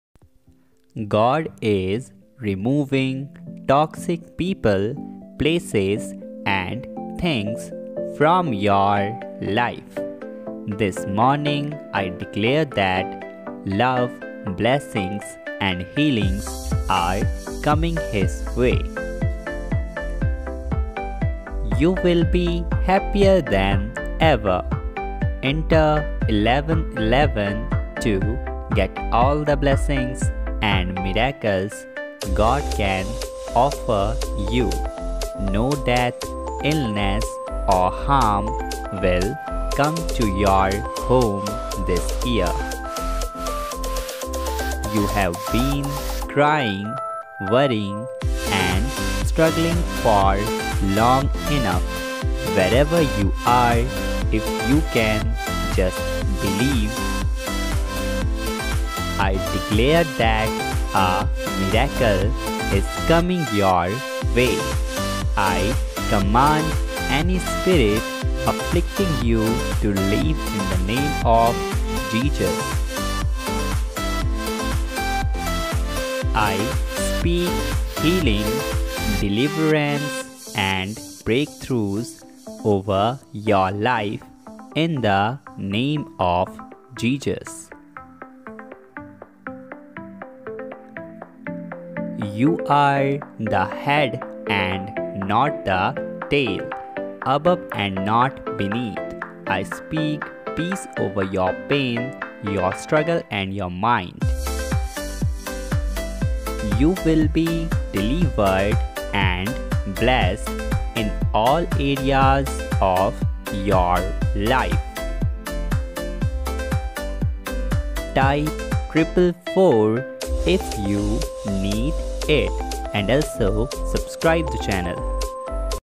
"The Ultimate Truth: God's Message for a Better Life" 👐😇❤️God's message for you 👐
God's messages are the source of hope, love, and inspiration that guide us through life's journey. From the holy scriptures to divine revelations, God's messages are timeless and hold valuable insights into living a fulfilling life. In this video, we explore the profound wisdom of God's messages and discover how they can transform our lives. Join us on a spiritual journey of discovery and unlock the power of God's messages for your life.